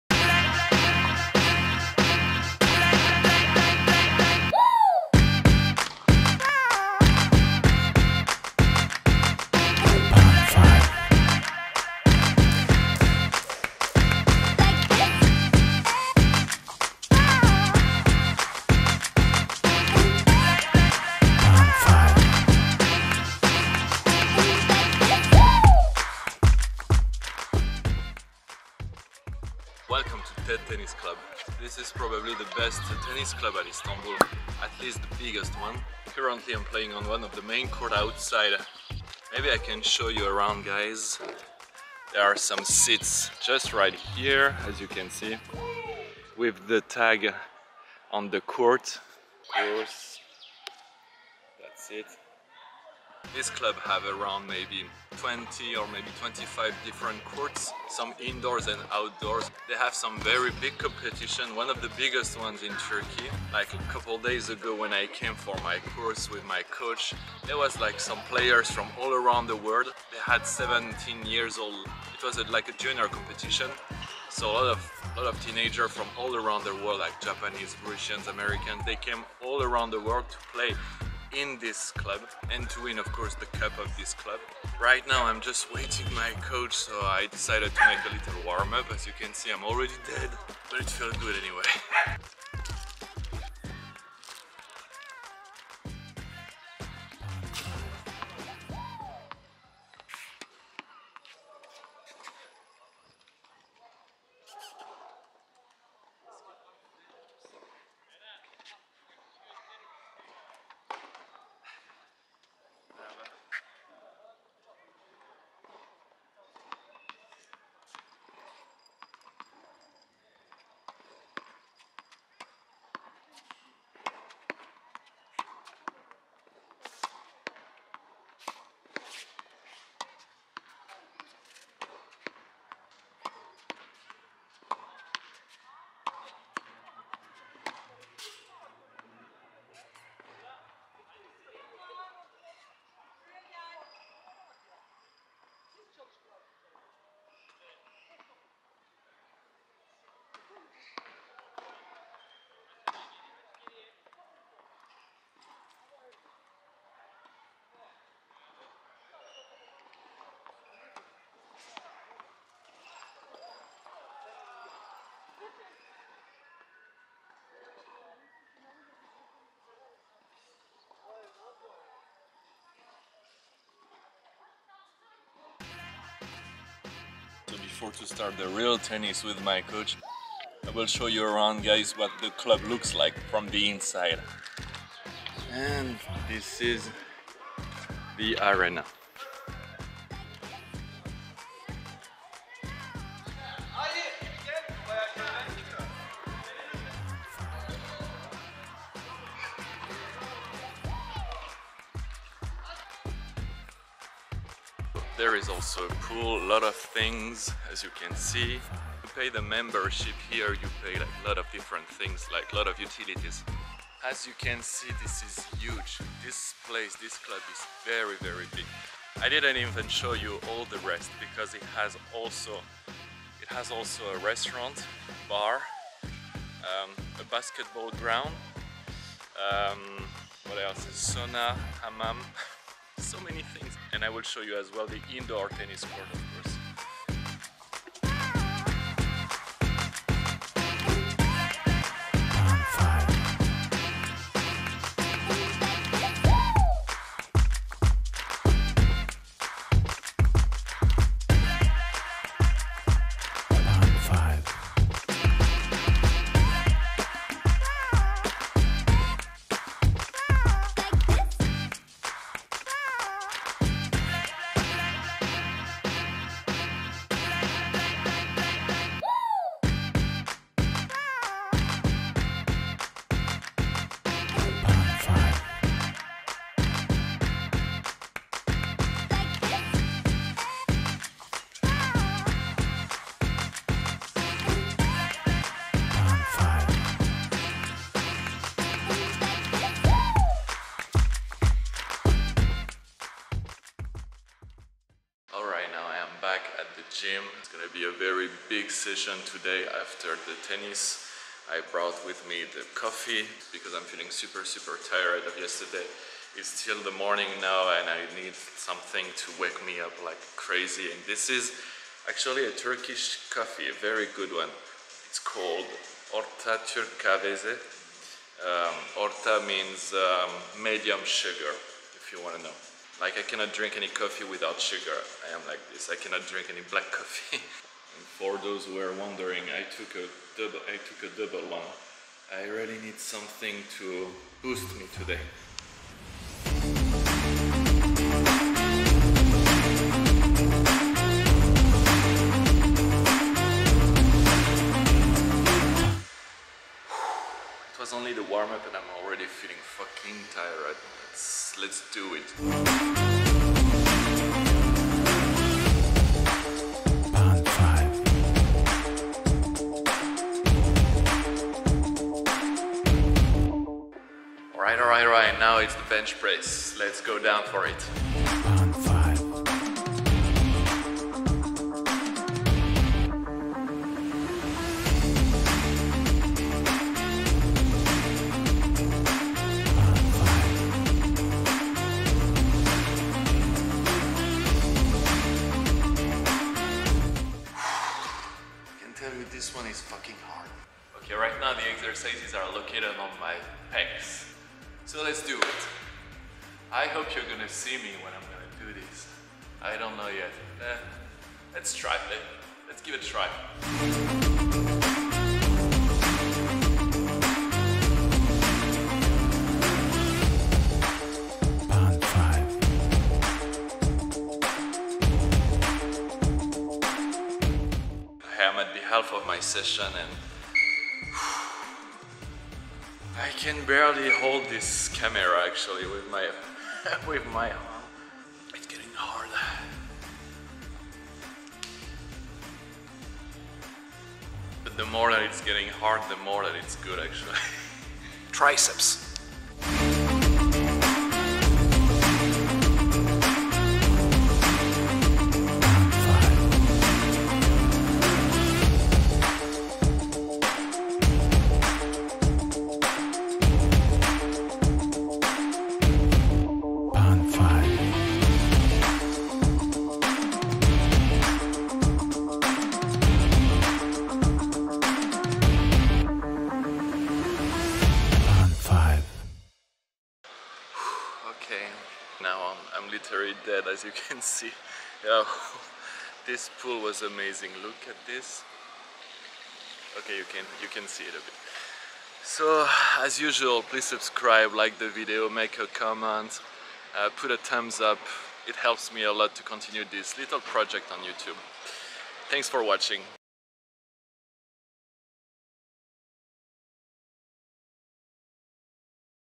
The BIGGEST TENNIS CLUB at ISTANBUL + Gym session @ home
Welcome to TED TENNIS CLUB. This is probably the best tennis club at Istanbul and in all the region. I will make a rapid tour of this amazing place, the facilities that you can find out there and i will show you my gym routine to get in shape before summer.

00:00 intro
00:27 Ted tenis kulubu
04:54 gym session
07:50 outro